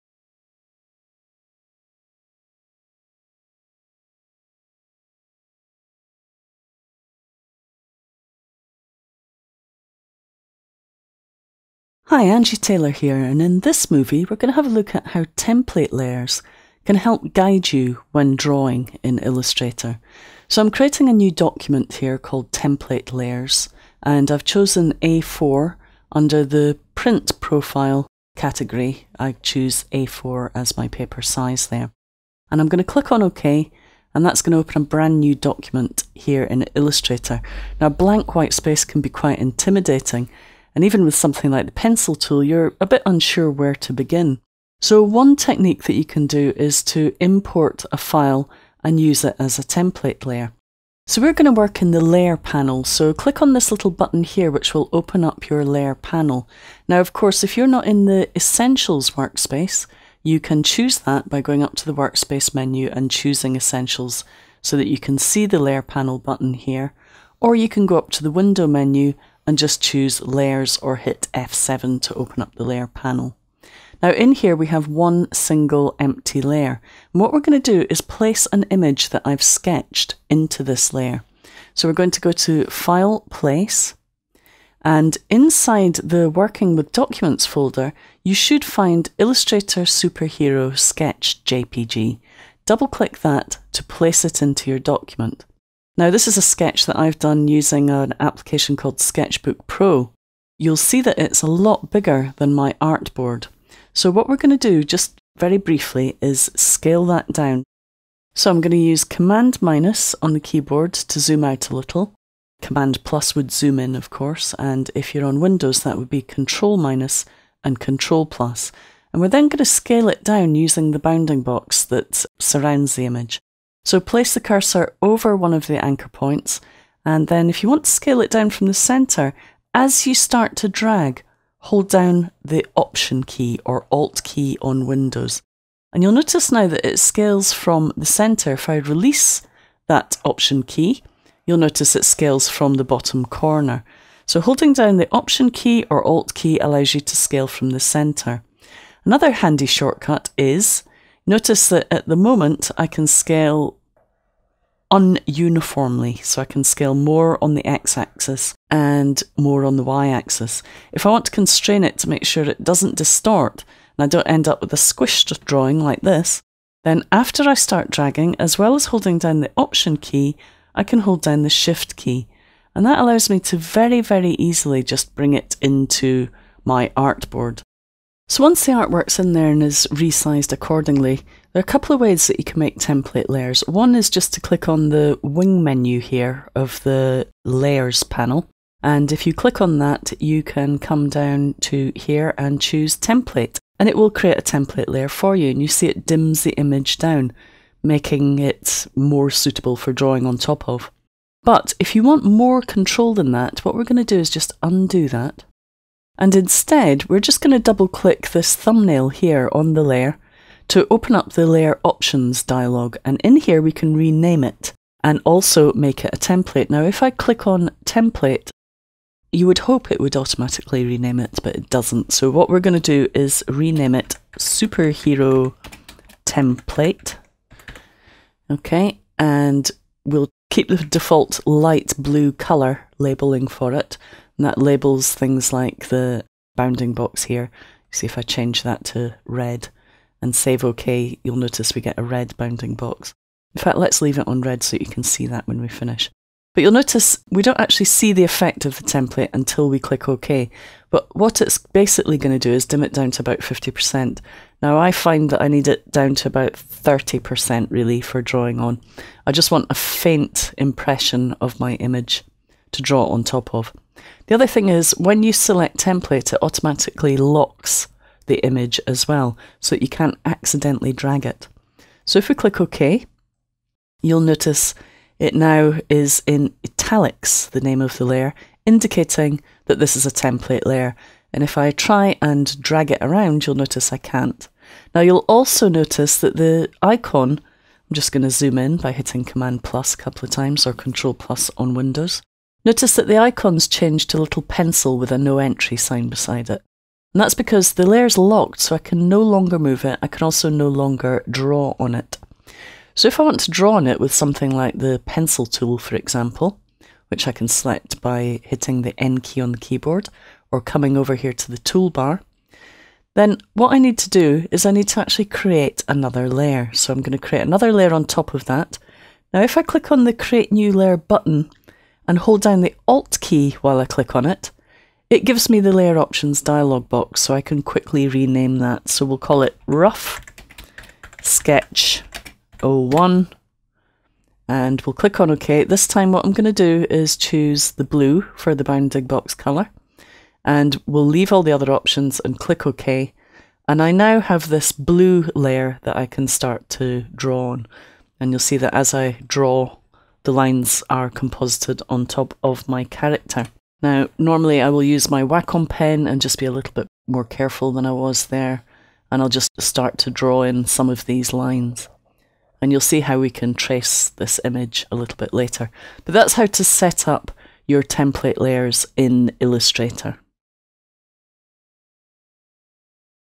Using Template Layers from: Adobe Illustrator CS6: Learn by Video
How to use Template layers in Adobe Illustrator to make drawing and tracing artwork a whole lot easier;